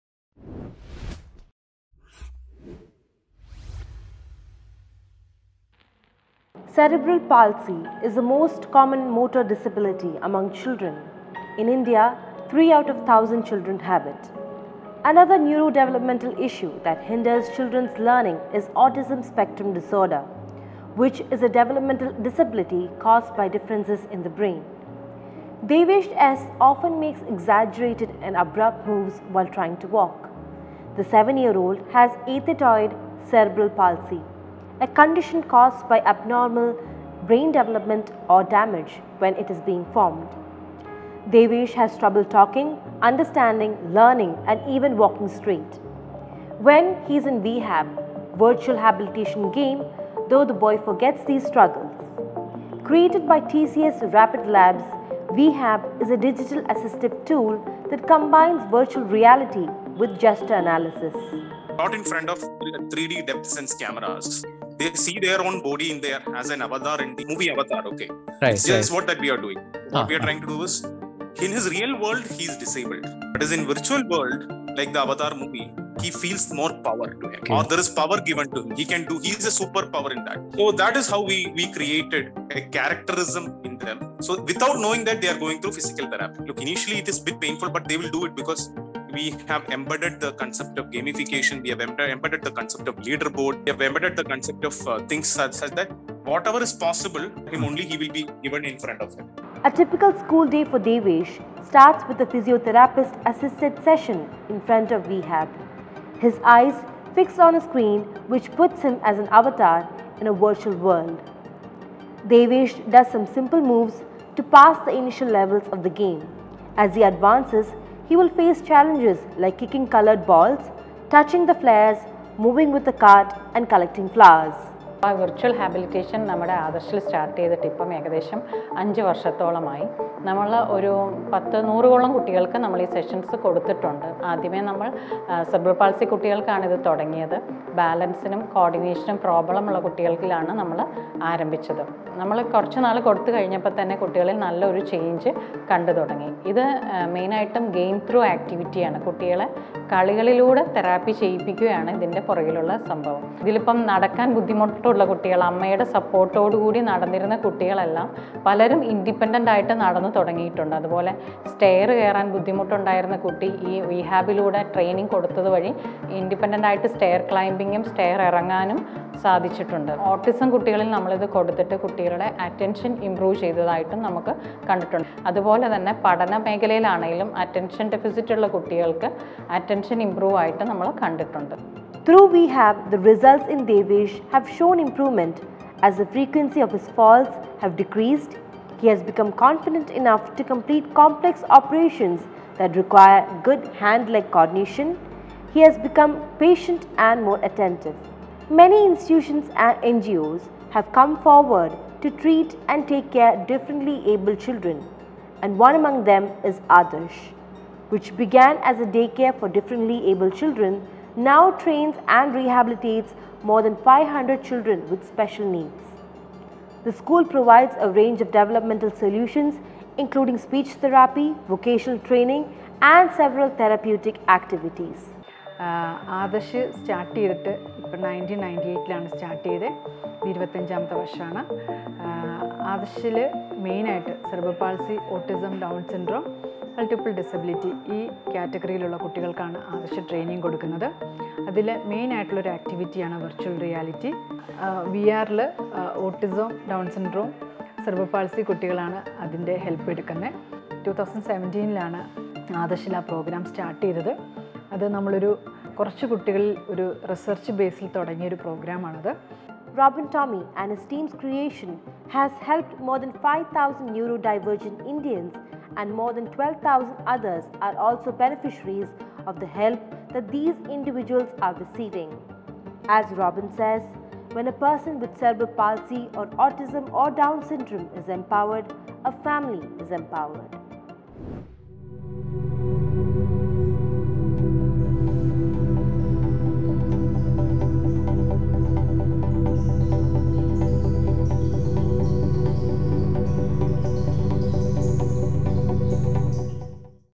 How VHAB is Helping the Neurodivergent | TCS Rapid Labs | THE WEEK
Cerebral palsy is the most common motor disability among children. VHAB (Virtual Habilitation) is digital-assertive tool that combines virtual reality with gesture analysis. The idea of VHAB came from James Cameron's 'Avatar'.